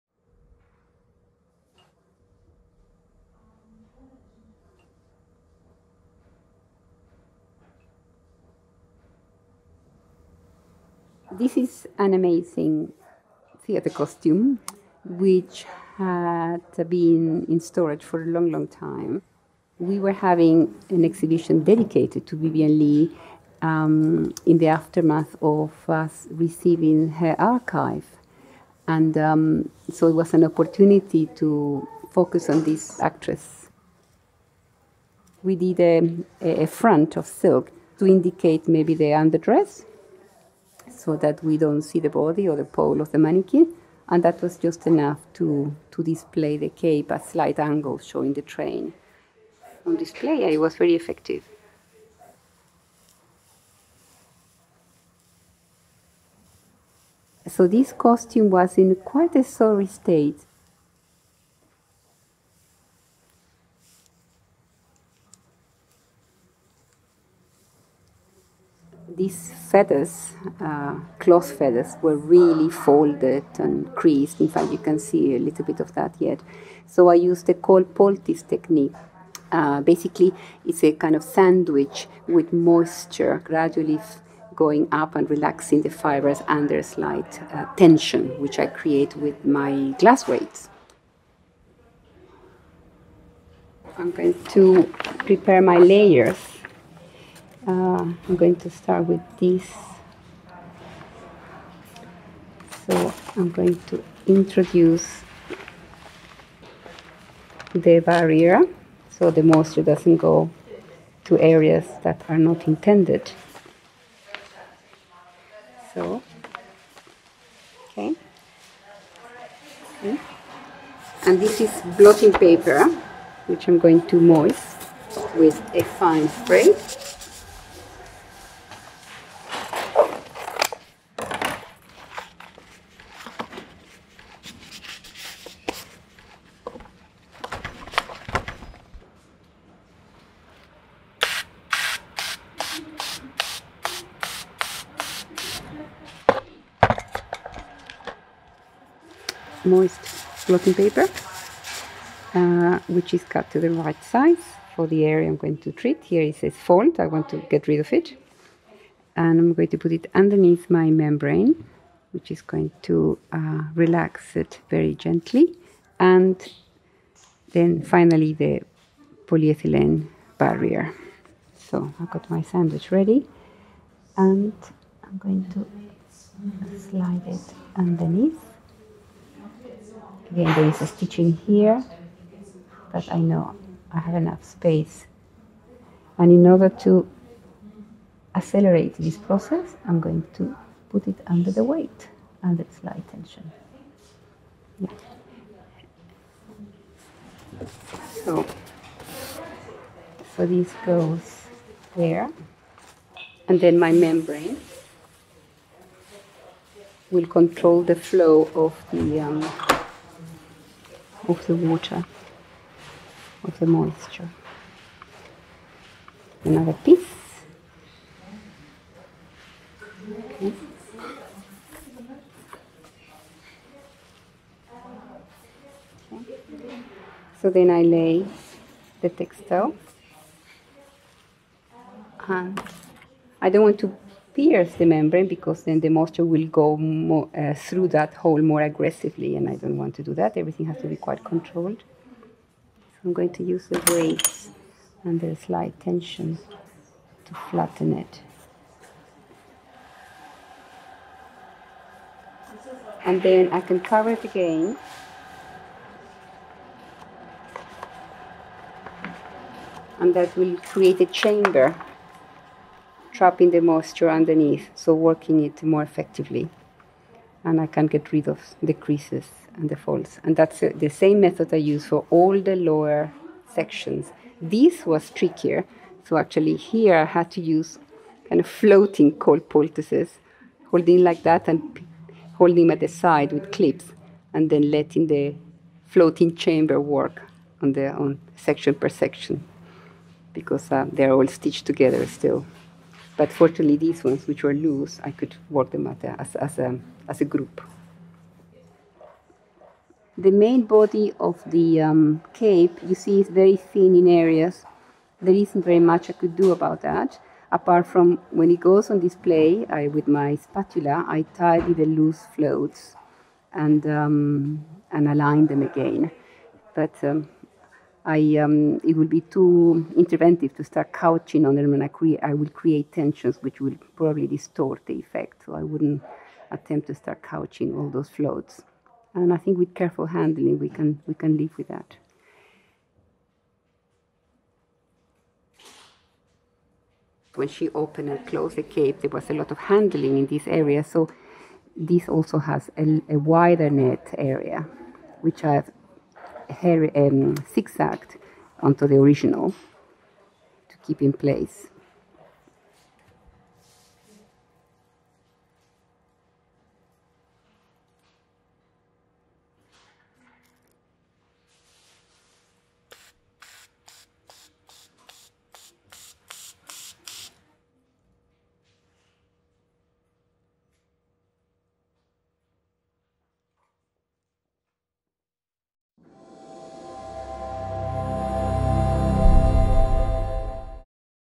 ASMR at the museum | Conserving Vivien Leigh's Cleopatra Cloak | V&A
// Listen with headphones for best experience //

Follow Senior Textile Conservator Susana Fajardo as she works on a cloak worn by Vivien Leigh for her performance as Cleopatra in Shakespeare's play 'Antony and Cleopatra', at the St. James's Theatre in 1951.

Sound designer and performance-maker Julie Rose Bower has documented Susana’s meticulous steps using ambisonic sound, letting us get closer to the object than ever before.

Watch as Susana uses the cold poultice technique to remove folds and creases in the cloth 'feathers' of the cloak, carefully utilising moisture and glass weights to relax the fibres.

Conservation process:
Examining the costume: 00:11
Explaining the cold poultice technique: 01:08
Preparing layers: 01:33
Cutting and spraying blotting paper: 2:00
Placing 'sandwiches' between the 'feathers' and adding the glass weights: 02:39
Creating a chamber to trap moisture: 04:12
Vivien Leigh wearing the cloak: 05:54